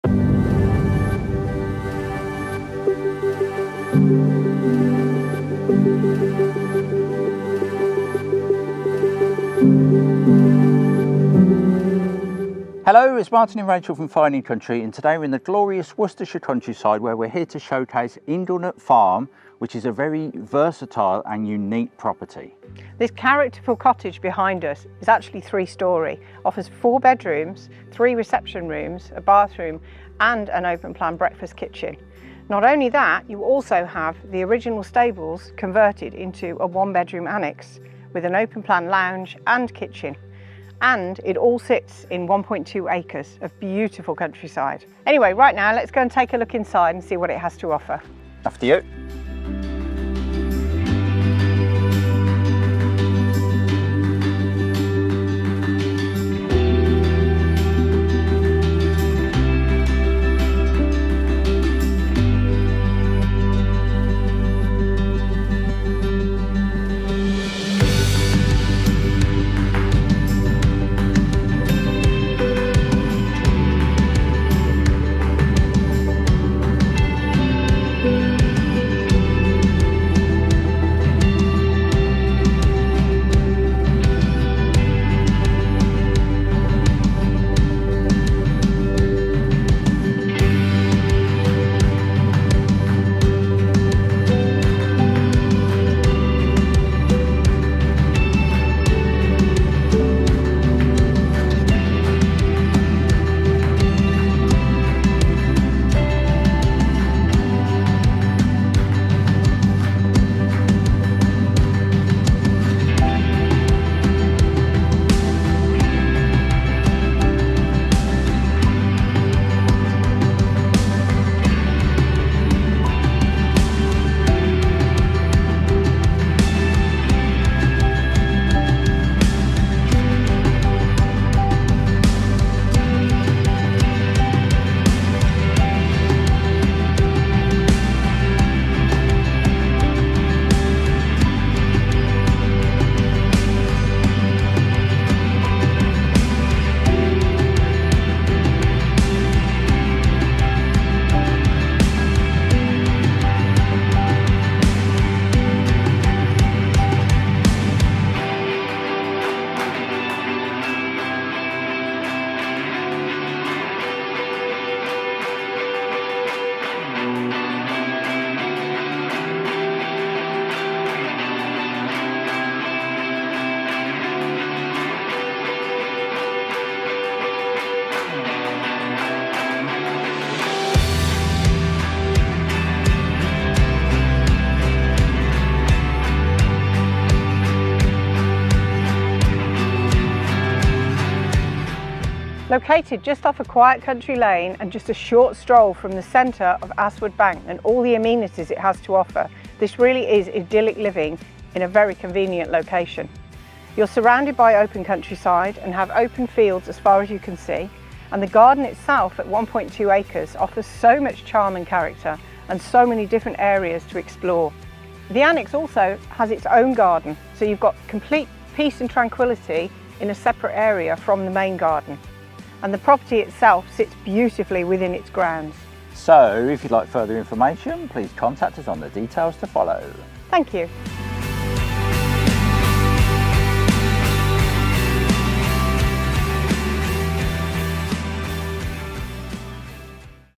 Astwood Lane, Astwood Bank, Worcestershire, UK - Fine & Country - Martin Grant and Rachel Hyde
Set back down a quiet country lane, just a short stroll from Astwood Bank village centre, Inglenook Farm is a delightfully charming three storey, four bedroom character farmhouse and fabulous separate fully fitted one bedroom annexe all sitting within circa 1.2 acres of beautiful gardens with unspoilt open countryside views.

Inglenook Farm, a character farmhouse dating back to 1937, has been lovingly maintained and underwent a complete renovation in the early 1980s. In 1988, a garage and stable block were added, later being transformed into a fabulous contemporary separate annexe. The main residence retains original features, including exposed wall and ceiling timbers, cornicing, and exposed brickwork in some rooms.

This charming and spacious home features an entrance hall, a dining room with a fireplace, a lounge with additional fireplace, a dual aspect sitting room offering garden and field views, and access to the beautiful grounds. The country-style breakfast kitchen includes a range cooker, with a utility room, downstairs WC, and a lean-to. Heading upstairs to the first floor landing you’ll find two bright and spacious double bedrooms with fitted wardrobes, a single bedroom, and a family bathroom. On the top floor, there's a fourth double bedroom with built-in wardrobes and exposed ceiling beams.

The separate annexe offers open-plan living, including a breakfast kitchen with an excellent range of floor and wall-mounted cabinets, built-in appliances, and a living room with a feature fireplace. There is a spacious double bedroom with a generous walk-in wardrobe and a well-appointed bathroom with a modern suite and a double shower. The Annexe has its own private enclosed garden, complete with a large, covered terrace and a gravelled courtyard. Connected to the annexe is a double garage with stairs leading to the first floor, where a substantial storage area offers potential for further conversion (STPP).

Outside
The property sits behind a private gated entrance leading to a sweeping gravelled driveway offering generous parking for multiple vehicles.  There is mature hedging, trees, and shrubbery to the front, creating a pretty ornamental fore garden.  The property very much sits within its plot, with outdoor space wrapping around three sides of Inglenook Farm offering circa 1.2 acres of beautiful formal and cottage gardens.  The property's spacious lawn is adorned with mature trees and shrubs, complemented by low wooden fencing that forms a natural, unobstructed boundary, revealing open fields and unspoiled views. Multiple terraces at the rear of the property offer a variety of options for outdoor entertainment and al fresco dining during the summer months, in this most idyllic location.

ASTWOOD BANK
The property sits right on the edge of the village with the wonderful Worcestershire countryside literally on its doorstep, affording great opportunities for walking and cycling. Astwood Bank is a village near the Worcestershire-Warwickshire border. The village is a thriving community, well served by grocery stores, post office, an award-winning butcher, cafés, public houses, a farm shop, doctor’s surgery and pharmacy. The Astwood Bank Community Group supports local businesses, organisations and enhances the local environment through fund-raising events and initiatives including the annual Carnival and Procession, Carols in the Park, Christmas lights and Astwood Bank in Bloom. A variety of clubs and societies are active ranging from the Operatic Society to local Scout and Guiding groups. The village is also noted for its successful cricket team who have twice played at Lords in the National Knock Out Final.

The M5 from J7 takes you to the Southwest, and to get to central Birmingham you would either cross the M42 at Junctions 2 or 3, or access the M42 at that point to get to other parts of the city. To travel North you would take the M5 at junction 5. All of these motorway junctions are c 15 minutes away, making fast routes to all points of the compass easily accessible, whilst the house itself is in a peaceful location unaffected by traffic noise.

The Property is Freehold |  EPC Rating: E | Council Tax Band: F

Fine & Country Solihull, Knowle & Dorridge

Martin Grant & Rachel Hyde | Partner Agents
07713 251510 or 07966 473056